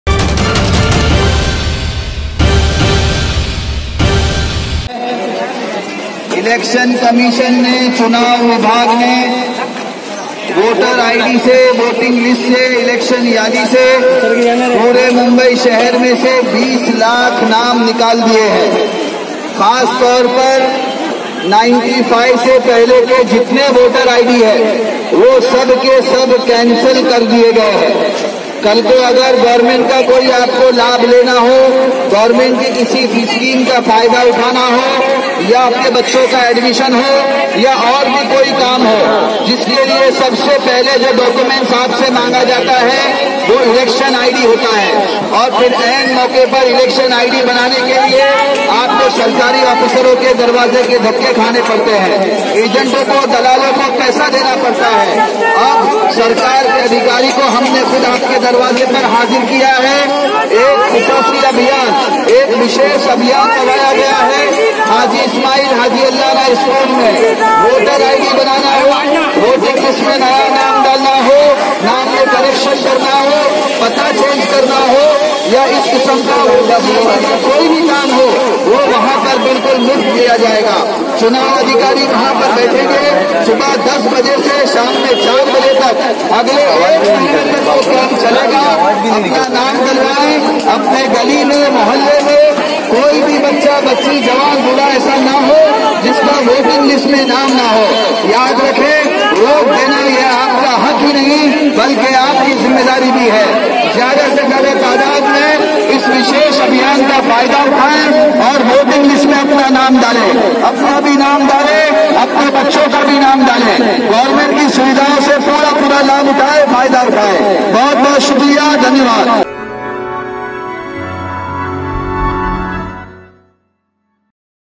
मुंबई में २० लाख़ मतदाताओं के नाम वोटर लिस्ट से निकाल दिए गये हैं सुफ़यान नियाज़ वाणु नगरसेवक कांग्रेस
मुंबई में २० लाख़ मतदाताओं के नाम वोटर लिस्ट से निकाल दिए गये हैं सुफ़यान नियाज़ वाणु नगर सेवक कांग्रेस, मतदाता  नामांकन जागरुकता अभियान
Voter Enrollment Awareness Campaign.